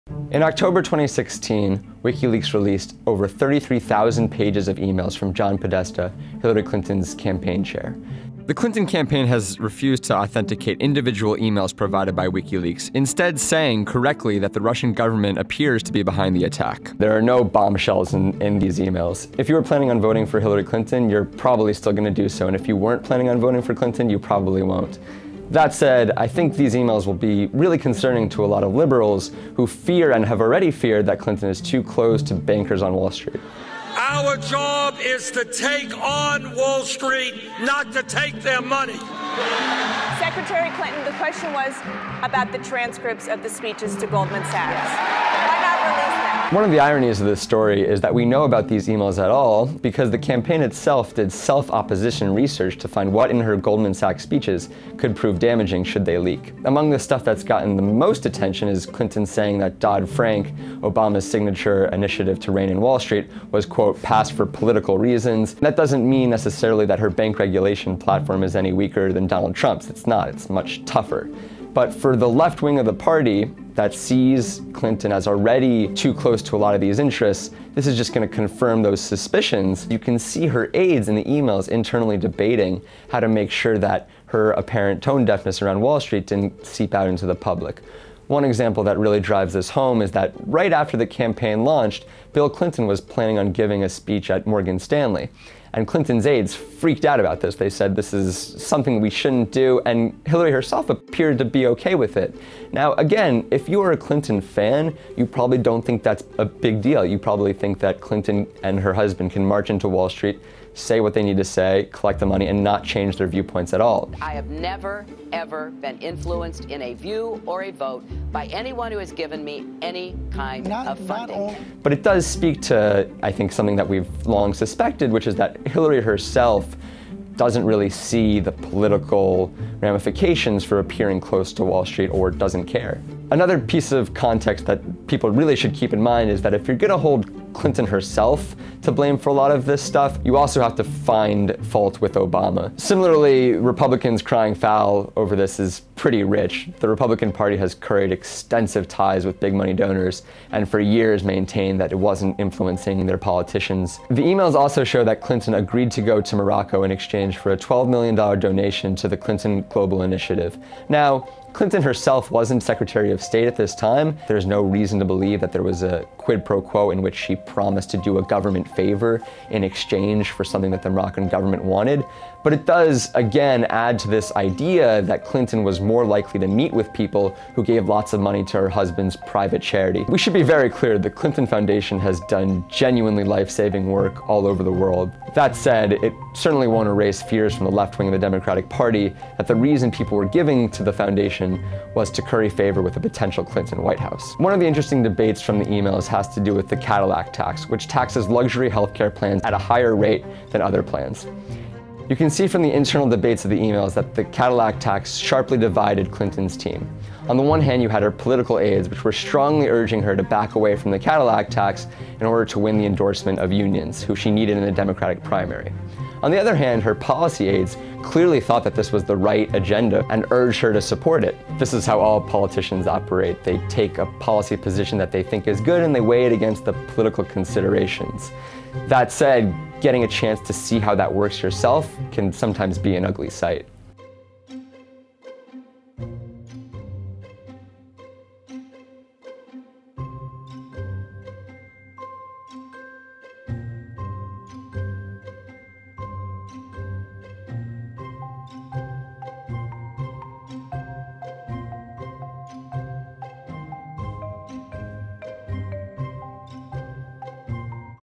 What 33,000 pages of leaked emails teach us about Hillary Clinton
No bombshells here. But it's worth a peek behind the curtain.